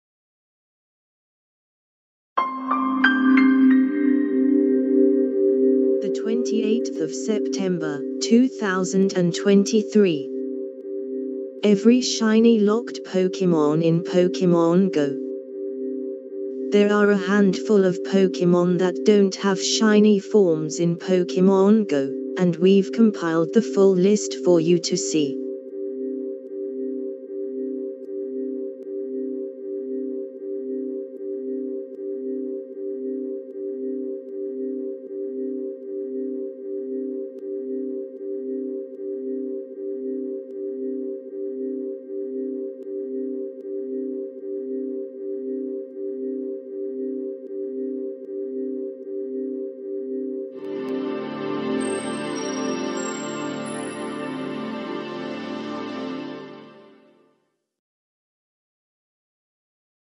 [Latest]Every Shiny Locked Pokemon in Pokemon Go (September 2023)
[Contents]
There are a handful of Pokemon that don't have shiny forms in Pokemon Go, and we've compiled the full list for you to see.